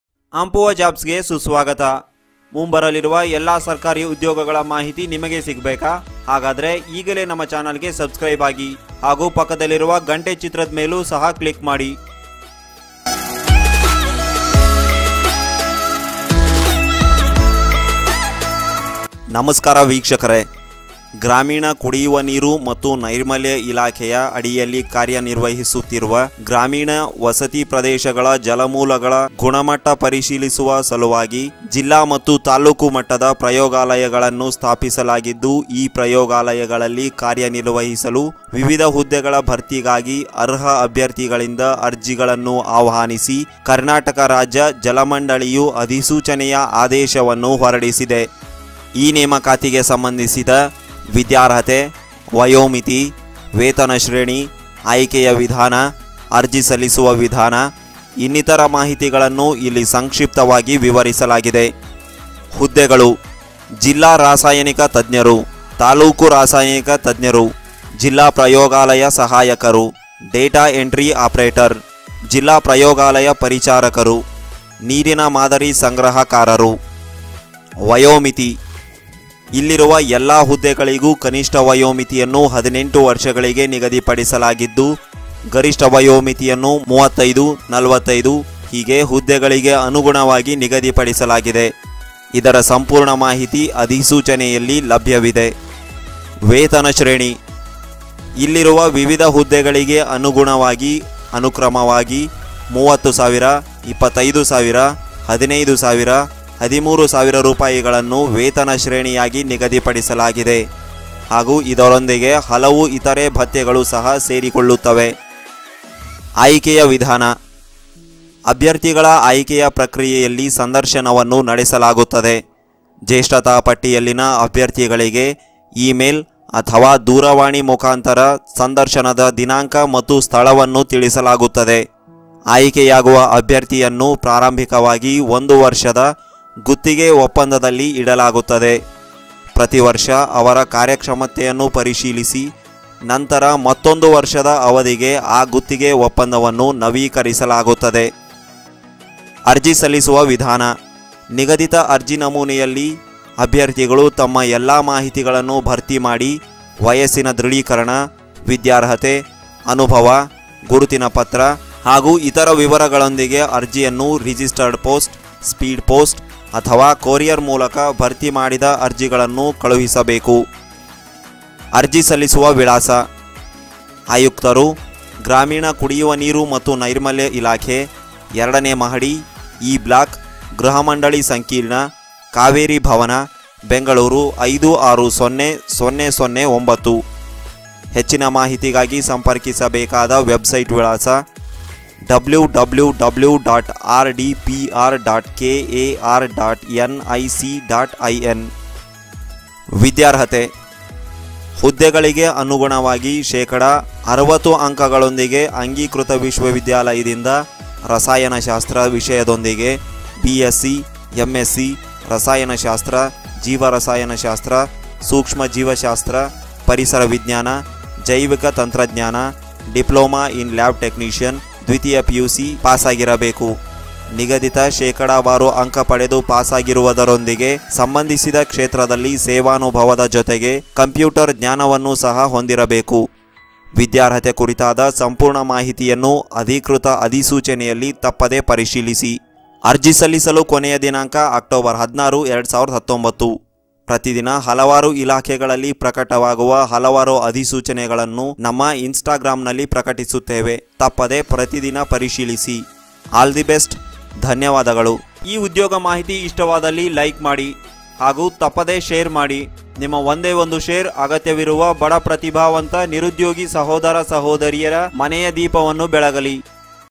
ಗ್ರಾಮೀಣ ಕುಡಿಯುವ ನೀರು ಮತ್ತು ನೈರ್ಮಲ್ಯ ಇಲಾಖೆಯ ನೇಮಕಾತಿ
ಈ ನೇಮಕಾತಿಗೆ ಓದಲೇಬೇಕಾದ ಪುಸ್ತಕ 👇👇
Hello viewers
 District and Taluk level laboratories have been set up to check the quality of water in rural residential areas functioning under the Rural Drinking Water and Sanitary Department.  Eligibility criteria for this assignment The method of applying for age limit pay scheme and other information are briefly described herein.
  * Posts *
 District Chemists
 Taluk Chemists
 District Laboratory Assistant
 Data entry operator
 District Laboratory Servicemen
 Water sample collectors.
  * Age group *
 The minimum age limit for all the posts here is 18 years and maximum age limit is 35/45.  Full information on this is available in the notification.
  * Salary range *
 Depending on the various posts, the salary range is 30000, 25000, 15000 and 13000 rupees respectively.  And a number of other allowances.
  * Method of Choice *
 Interviews are conducted in the selection process of candidates. The candidates on the seniority list will be notified via email or telephone.
 The elected candidate will initially be placed on a one-year lease agreement, which will review their performance each year and then renew that contract for another year.
  * Method of Application *
 Candidates must fill out all their information in the prescribed application form and send the application with Registered Post / Speed ​​Post or Courier along with age confirmation, qualification, experience, identity card and other details.
  * Application Address *
 Commissioner,
 Department of Rural Drinking Water and Sanitation,
 Second floor,
  E Block,
 Home House complex,
 Kaveri Bhawan,
 Bangalore-560 009.
  * Qualifications *.
 BSc M.Sc in Chemistry / Biochemistry / Microbiology / Ecology / Biotechnology / Diploma in Lab Technician / Secondary PUC with Chemistry Degree from recognized University with 60% marks.  Must have computer knowledge as well as computer knowledge in the field related to passing the specified percentage.  Be sure to check the full information on qualifications in the official notification.
 The deadline to apply is October 16 2019.
 We post a number of notifications on our Instagram every day.
ನಮಸ್ಕಾರ ವೀಕ್ಷಕರೇ
ಗ್ರಾಮೀಣ ಕುಡಿಯುವ ನೀರು ಮತ್ತು ನೈರ್ಮಲ್ಯ ಇಲಾಖೆಯ ಅಡಿಯಲ್ಲಿ ಕಾರ್ಯನಿರ್ವಹಿಸುತ್ತಿರುವ ಗ್ರಾಮೀಣ ವಸತಿ ಪ್ರದೇಶಗಳ ಜಲಮೂಲಗಳ ಗುಣಮಟ್ಟ ಪರೀಕ್ಷಿಸುವ ಸಲುವಾಗಿ ಜಿಲ್ಲಾ ಮತ್ತು ತಾಲ್ಲೂಕು ಮಟ್ಟದ ಪ್ರಯೋಗಾಲಯಗಳನ್ನು ಸ್ಥಾಪಿಸಲಾಗಿದ್ದು ಈ ಪ್ರಯೋಗಾಲಯಗಳಲ್ಲಿ ಕಾರ್ಯನಿರ್ವಹಿಸಲು ವಿವಿಧ ಹುದ್ದೆಗಳ ಭರ್ತಿಗಾಗಿ ಅರ್ಹ ಅಭ್ಯರ್ಥಿಗಳಿಂದ ಅರ್ಜಿಗಳನ್ನು ಆಹ್ವಾನಿಸಿ ಕರ್ನಾಟಕ ರಾಜ್ಯ ಜಲಮಂಡಳಿಯು ಅಧಿಸೂಚನೆಯ ಆದೇಶವನ್ನು ಹೊರಡಿಸಿದೆ. ಈ ನೇಮಕಾತಿಗೆ ಸಂಬಂಧಿಸಿದ ವಿದ್ಯಾರ್ಹತೆ ವಯೋಮಿತಿ ವೇತನಶ್ರೇಣಿ ಆಯ್ಕೆಯ ವಿಧಾನ ಅರ್ಜಿ ಸಲ್ಲಿಸುವ ವಿಧಾನ ಇನ್ನಿತರ ಮಾಹಿತಿಗಳನ್ನು ಇಲ್ಲಿ ಸಂಕ್ಷಿಪ್ತವಾಗಿ ವಿವರಿಸಲಾಗಿದೆ.
 *ಹುದ್ದೆಗಳು*
ಜಿಲ್ಲಾ ರಾಸಾಯನಿಕ ತಜ್ಞರು
ತಾಲೂಕು ರಾಸಾಯನಿಕ ತಜ್ಞರು
ಜಿಲ್ಲಾ ಪ್ರಯೋಗಾಲಯ ಸಹಾಯಕರು
ಡೇಟಾ ಎಂಟ್ರಿ ಆಪರೇಟರ್
ಜಿಲ್ಲಾ ಪ್ರಯೋಗಾಲಯ ಪರಿಚಾರಕರು
ನೀರಿನ ಮಾದರಿ ಸಂಗ್ರಹಕಾರರು.
 *ವಯೋಮಿತಿ*
ಇಲ್ಲಿರುವ ಎಲ್ಲಾ ಹುದ್ದೆಗಳಿಗೂ ಕನಿಷ್ಠ ವಯೋಮಿತಿಯನ್ನು 18 ವರ್ಷಗಳಿಗೆ ನಿಗದಿಪಡಿಸಲಾಗಿದ್ದು ಗರಿಷ್ಠ ವಯೋಮಿತಿಯನ್ನು 35/45 ಹೀಗೆ ಹುದ್ದೆಗಳಿಗೆ ಅನುಗುಣವಾಗಿ ನಿಗದಿಪಡಿಸಲಾಗಿದೆ. ಇದರ ಸಂಪೂರ್ಣ ಮಾಹಿತಿ ಅಧಿಸೂಚನೆಯಲ್ಲಿ ಲಭ್ಯವಿದೆ .
 *ವೇತನ ಶ್ರೇಣಿ*
ಇಲ್ಲಿರುವ ವಿವಿಧ ಹುದ್ದೆಗಳಿಗೆ ಅನುಗುಣವಾಗಿ ಅನುಕ್ರಮವಾಗಿ 30000, 25000, 15000, 13000 ರೂಪಾಯಿಗಳನ್ನು ವೇತನ ಶ್ರೇಣಿ ಯಾಗಿ ನಿಗದಿಪಡಿಸಲಾಗಿದೆ. ಹಾಗೂ ಇದರೊಂದಿಗೆ ಹಲವು ಇತರೆ ಭತ್ಯೆಗಳು ಸಹ ಸೇರಿಕೊಳ್ಳುತ್ತವೆ.
 *ಆಯ್ಕೆಯ ವಿಧಾನ*
ಅಭ್ಯರ್ಥಿಗಳ ಆಯ್ಕೆಯ ಪ್ರಕ್ರಿಯೆಯಲ್ಲಿ ಸಂದರ್ಶನವನ್ನು ನಡೆಸಲಾಗುತ್ತದೆ ಜೇಷ್ಠತಾ ಪಟ್ಟಿಯಲ್ಲಿನ ಅಭ್ಯರ್ಥಿಗಳಿಗೆ ಇಮೇಲ್ ಅಥವಾ ದೂರವಾಣಿ ಮುಖಾಂತರ ಸಂದರ್ಶನದ ದಿನಾಂಕ ಮತ್ತು ಸ್ಥಳವನ್ನು ತಿಳಿಸಲಾಗುತ್ತದೆ.
ಆಯ್ಕೆಯಾಗುವ ಅಭ್ಯರ್ಥಿಯನ್ನು ಪ್ರಾರಂಭಿಕವಾಗಿ ಒಂದು ವರ್ಷದ ಗುತ್ತಿಗೆ ಒಪ್ಪಂದದಲ್ಲಿ ಇಡಲಾಗುತ್ತದೆ ಪ್ರತಿವರ್ಷ ಅವರ ಕಾರ್ಯಕ್ಷಮತೆಯನ್ನು ಪರಿಶೀಲಿಸಿ ನಂತರ ಮತ್ತೊಂದು ವರ್ಷದ ಅವಧಿಗೆ ಆ ಗುತ್ತಿಗೆ ಒಪ್ಪಂದವನ್ನು ನವೀಕರಿಸಲಾಗುತ್ತದೆ.
 *ಅರ್ಜಿ ಸಲ್ಲಿಸುವ ವಿಧಾನ*
ನಿಗದಿತ ಅರ್ಜಿ ನಮೂನೆಯಲ್ಲಿ ಅಭ್ಯರ್ಥಿಗಳು ತಮ್ಮ ಎಲ್ಲಾ ಮಾಹಿತಿಗಳನ್ನು ಭರ್ತಿ ಮಾಡಿ, ವಯಸ್ಸಿನ ದೃಢೀಕರಣ, ವಿದ್ಯಾರ್ಹತೆ, ಅನುಭವ, ಗುರುತಿನ ಪತ್ರ ಹಾಗೂ ಇತರ ವಿವರಗಳೊಂದಿಗೆ ಅರ್ಜಿಯನ್ನು ರಿಜಿಸ್ಟರ್ಡ್ ಪೋಸ್ಟ್/ ಸ್ಪೀಡ್ ಪೋಸ್ಟ್/ ಅಥವಾ ಕೋರಿಯರ್ ಮೂಲಕ ಭರ್ತಿ ಮಾಡಿದ ಅರ್ಜಿಗಳನ್ನು ಕಳುಹಿಸಬೇಕು.
 *ಅರ್ಜಿ ಸಲ್ಲಿಸುವ ವಿಳಾಸ*
ಆಯುಕ್ತರು,
ಗ್ರಾಮೀಣ ಕುಡಿಯುವ ನೀರು ಮತ್ತು ನೈರ್ಮಲ್ಯ ಇಲಾಖೆ,
ಎರಡನೇ ಮಹಡಿ,
 E ಬ್ಲಾಕ್ ,
ಗೃಹಮಂಡಳಿ ಸಂಕೀರ್ಣ,
ಕಾವೇರಿ ಭವನ,
ಬೆಂಗಳೂರು-560 009.
ಹೆಚ್ಚಿನ ಮಾಹಿತಿಗಾಗಿ ಸಂಪರ್ಕಿಸಬೇಕಾದ ವೆಬ್ ಸೈಟ್ ವಿಳಾಸ
 *ವಿದ್ಯಾರ್ಹತೆ* .
ಹುದ್ದೆಗಳಿಗೆ ಅನುಗುಣವಾಗಿ ಶೇಕಡಾ 60 ಅಂಕಗಳೊಂದಿಗೆ ಅಂಗೀಕೃತ ವಿಶ್ವವಿದ್ಯಾಲಯದಿಂದ ರಸಾಯನಶಾಸ್ತ್ರ ವಿಷಯದೊಂದಿಗೆ ಬಿಎಸ್ಸಿ ಎಂಎಸ್ಸಿ ರಸಾಯನಶಾಸ್ತ್ರ/ಜೀವರಸಾಯನಶಾಸ್ತ್ರ/ ಸೂಕ್ಷ್ಮಜೀವಶಾಸ್ತ್ರ/ಪರಿಸರವಿಜ್ಞಾನ/ ಜೈವಿಕತಂತ್ರಜ್ಞಾನ/ ಡಿಪ್ಲೋಮಾ ಇನ್ ಲ್ಯಾಬ್ ಟೆಕ್ನಿಷಿಯನ್/ದ್ವಿತೀಯ ಪಿಯುಸಿ ಪಾಸಾಗಿರಬೇಕು. ನಿಗದಿತ ಶೇಕಡಾವಾರು ಅಂಕ ಪಡೆದು ಪಾಸಾಗಿರುವುದರೊಂದಿಗೆ ಸಂಬಂಧಿಸಿದ ಕ್ಷೇತ್ರದಲ್ಲಿ ಸೇವಾನುಭವ ದ ಜೊತೆಗೆ ಕಂಪ್ಯೂಟರ್ ಜ್ಞಾನವನ್ನು ಸಹ ಹೊಂದಿರಬೇಕು. ವಿದ್ಯಾರ್ಹತೆ ಕುರಿತಾದ ಸಂಪೂರ್ಣ ಮಾಹಿತಿಯನ್ನು ಅಧಿಕೃತ ಅಧಿಸೂಚನೆಯಲ್ಲಿ ತಪ್ಪದೆ ಪರಿಶೀಲಿಸಿ.
ಅರ್ಜಿ ಸಲ್ಲಿಸಲು ಕೊನೆಯ ದಿನಾಂಕ ಅಕ್ಟೋಬರ್ 16 2019.
ಪ್ರತಿದಿನ ಹಲವಾರು ಇಲಾಖೆಗಳಲ್ಲಿ ಪ್ರಕಟವಾಗುವ ಹಲವಾರು ಅಧಿಸೂಚನೆಗಳನ್ನು ನಮ್ಮ ಇನ್ಸ್ಟಾಗ್ರಾಮ್ ನಲ್ಲಿ ಪ್ರಕಟಿಸುತ್ತೇವೆ ತಪ್ಪದೇ ಪ್ರತಿದಿನ ಪರಿಶೀಲಿಸಿ
ಧನ್ಯವಾದಗಳು